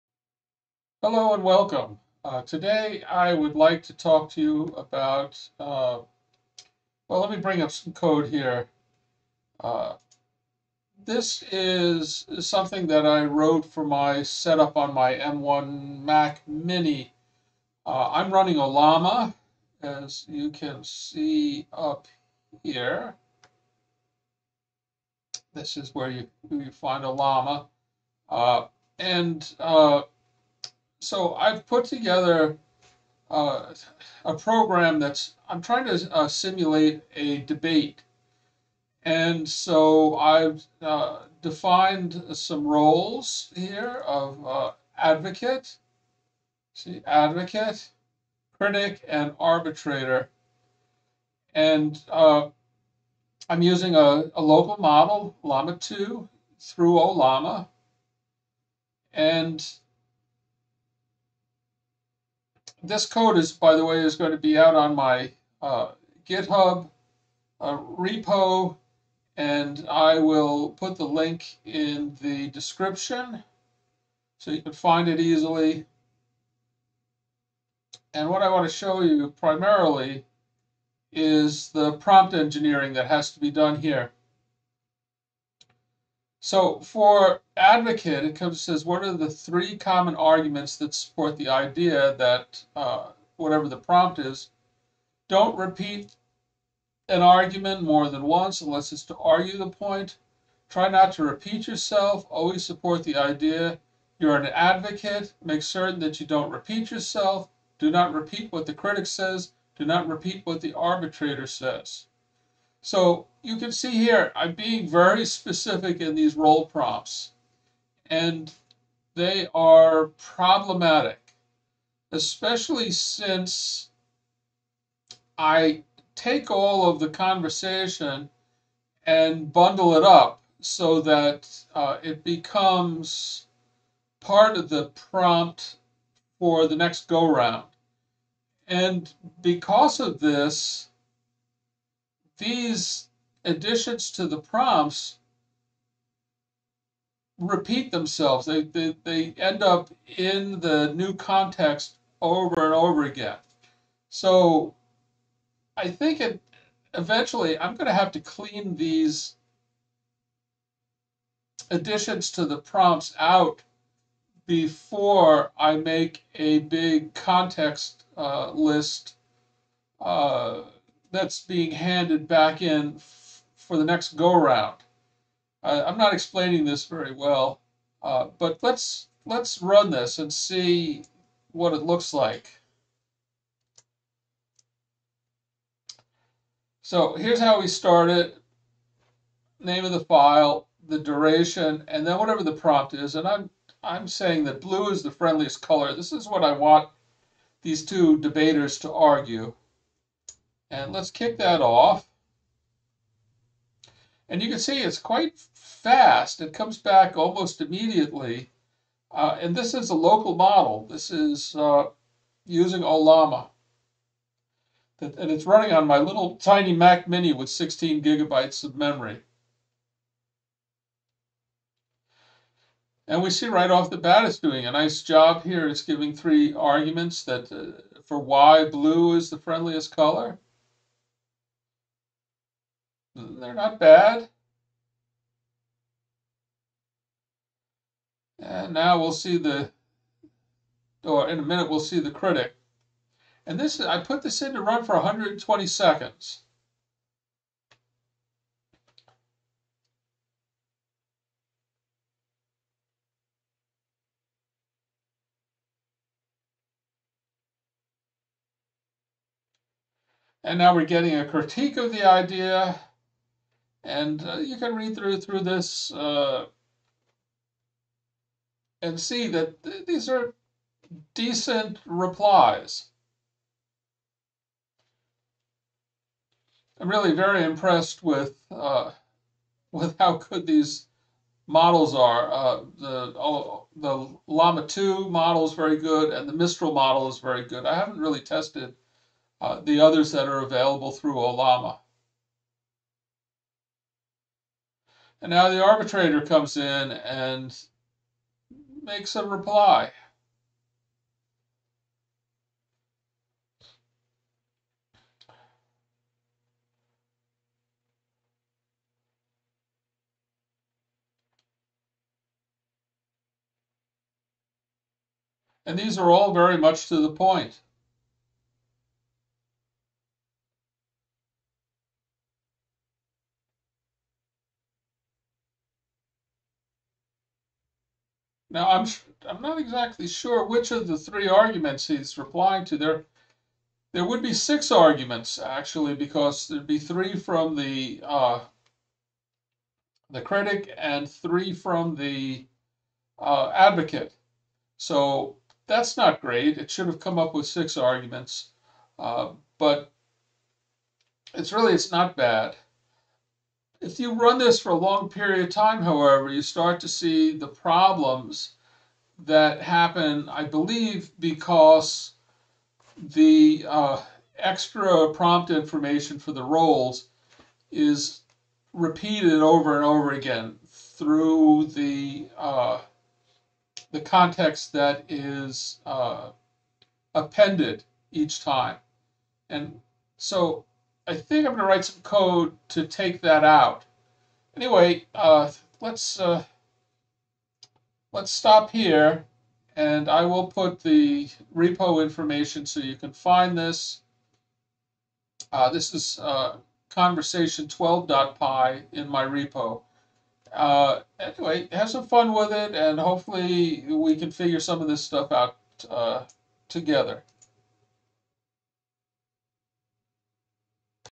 Two AI agents debating
This simple program uses a local (LLM) model running on Ollama on my 16GB MacMini.   Here is the repo